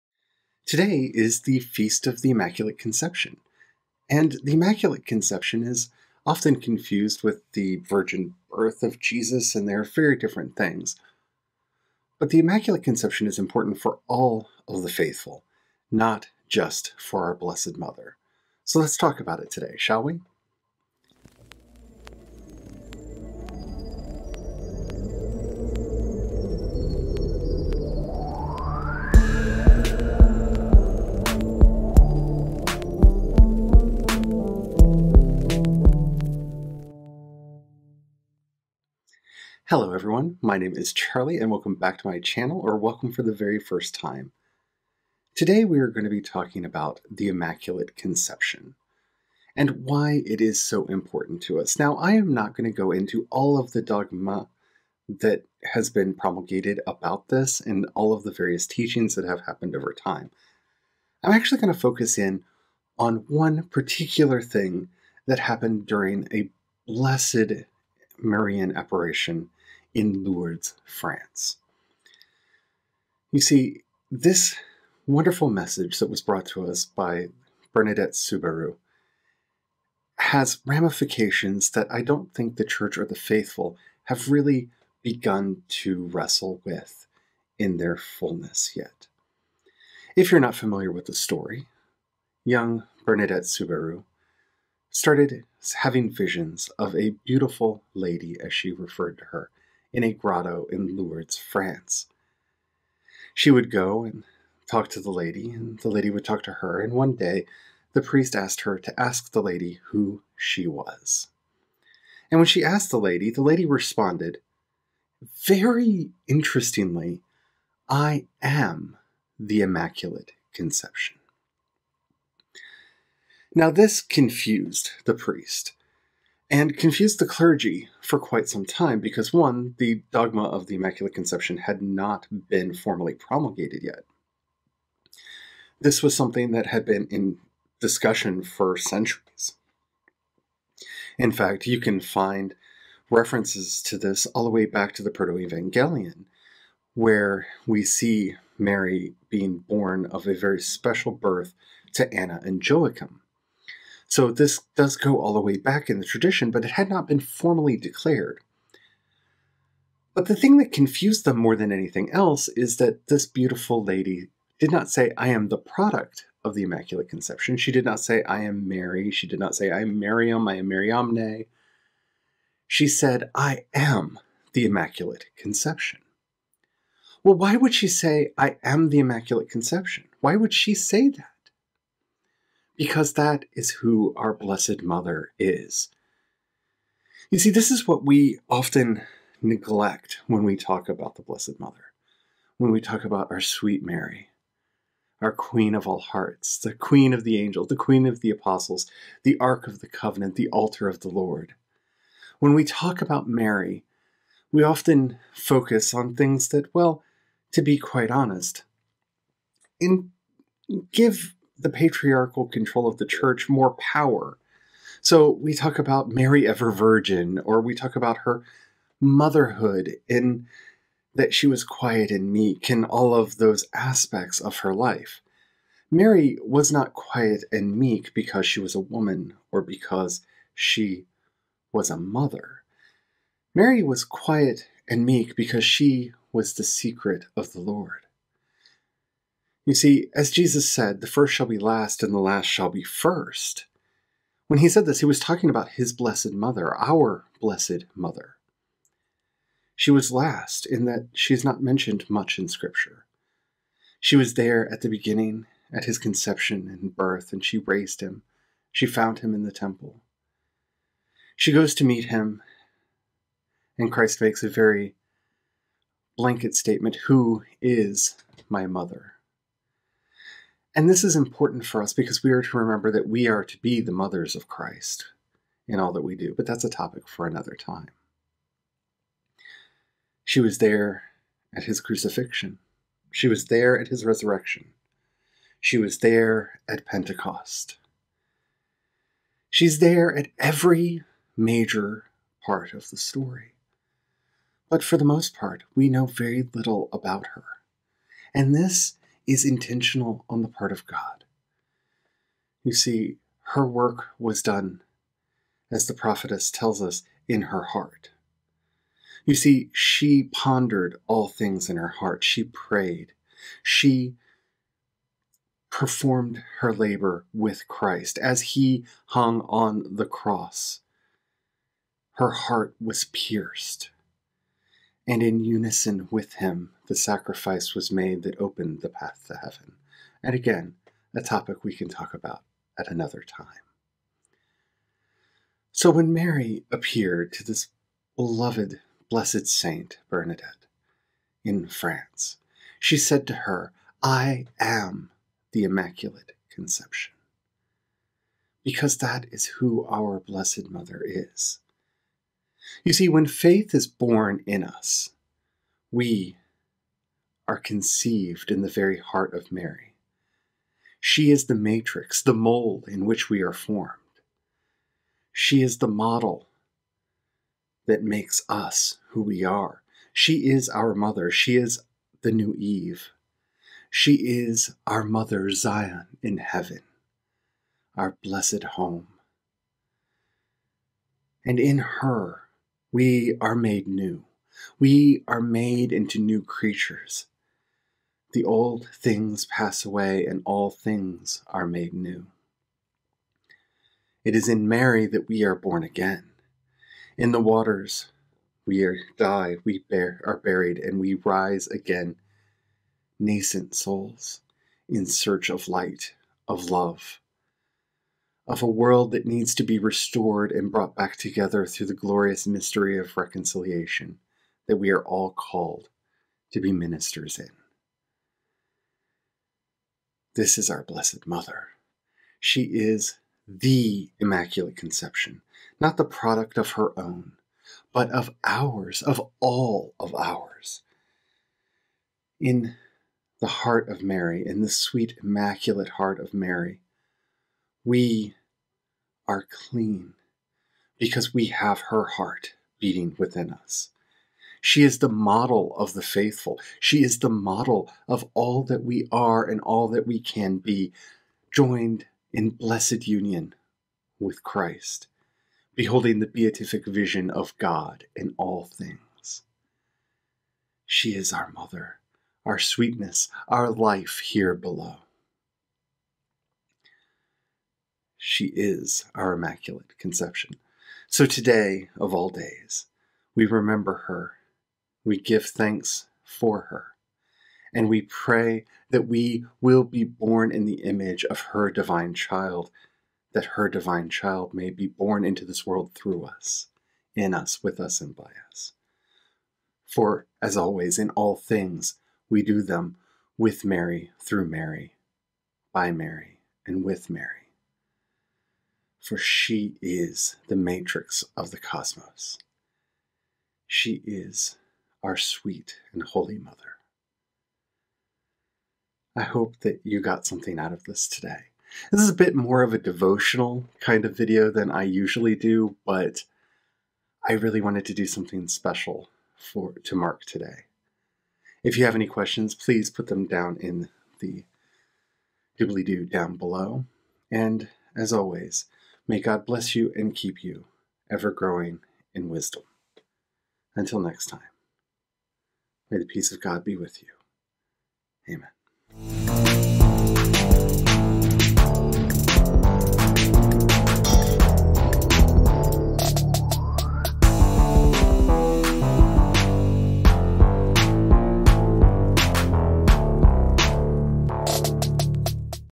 Mary, Our Blessed Mother is our Immaculate Conception
Let's talk about the ramifications of the visions of Saint Bernadette Soubirous at Lourdes, France, where she asked the Blessed Mother for her name and she answered, "I am the Immaculate Conception." What a blessing to all life.